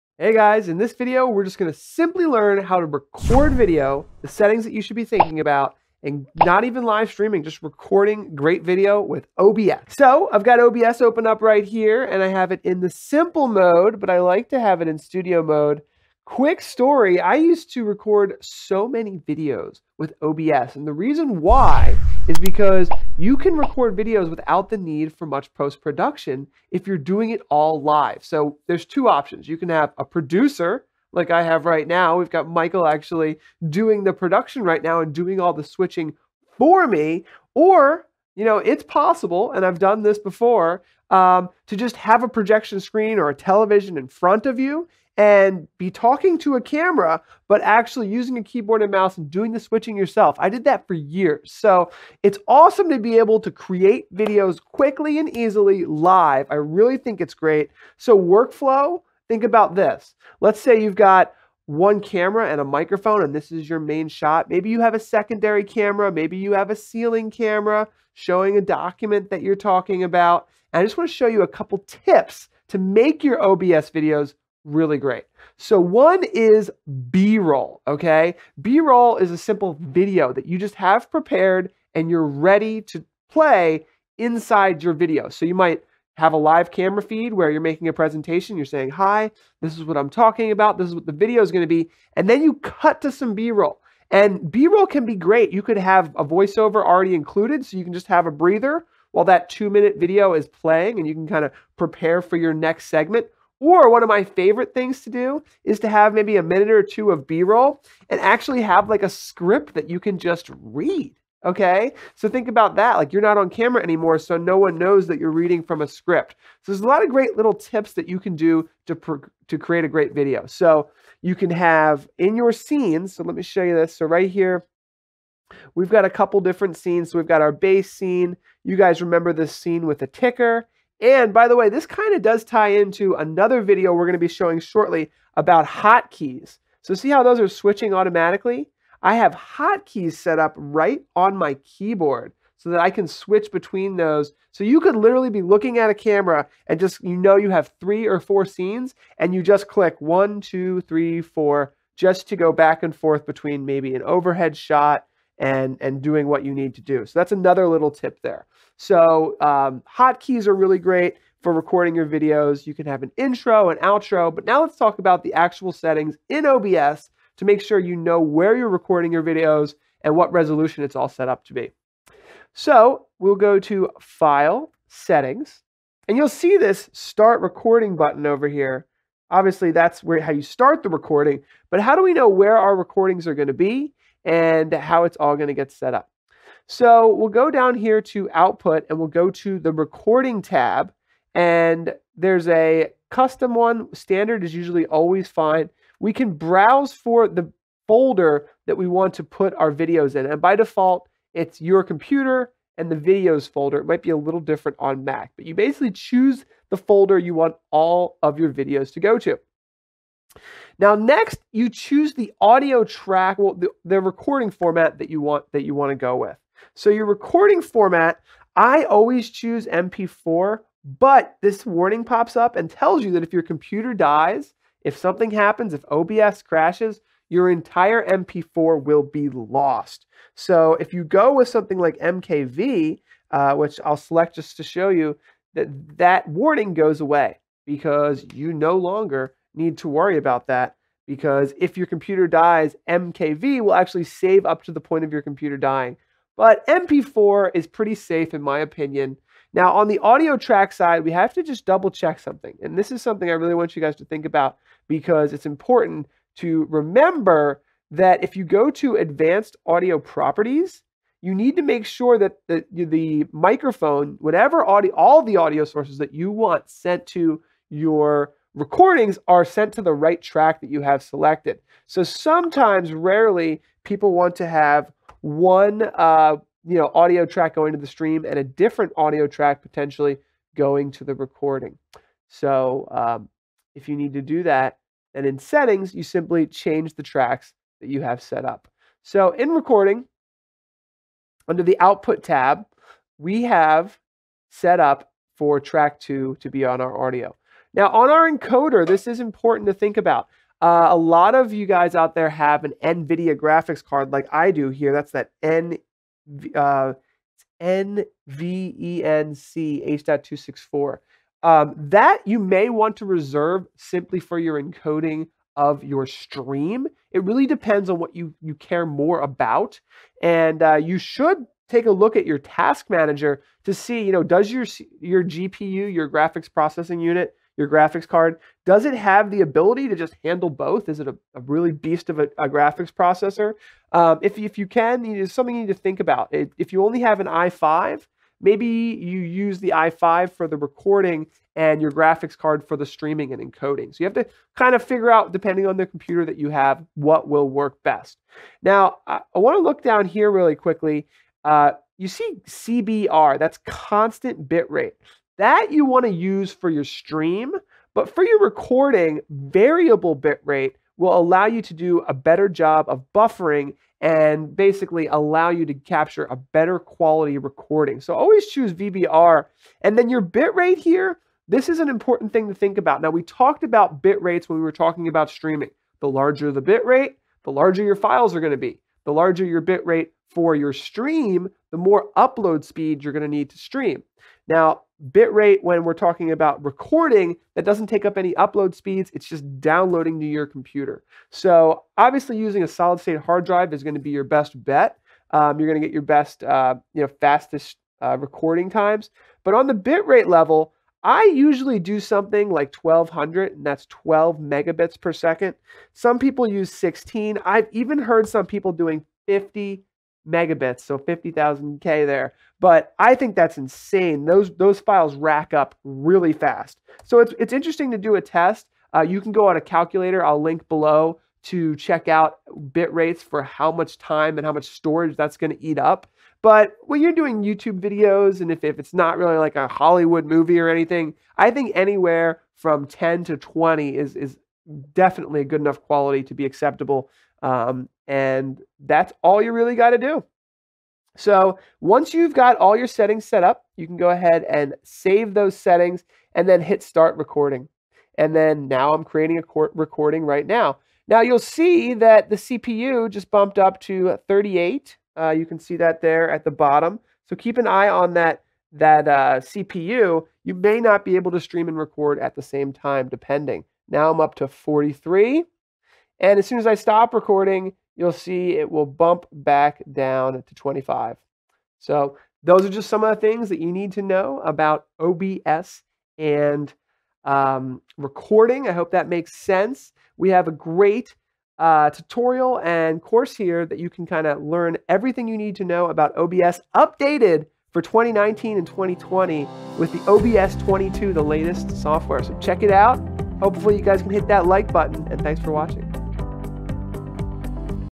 How to record videos in OBS - 2019 OBS Course
In this video, we walk you through how to use Open Broadcaster Software to record videos. One thing that a lot people don't realize about live video production is it's great ability to capture produced content. Depending on your level of skill many people can create live video content with a produced feel that is very similar to post-production video. Using live production software like OBS with multiple scenes and layouts, you will learn how to create dynamic video presentations. When done correctly you include intro videos, lower thirds, picture in picture layouts and more that will reduce your need for post production.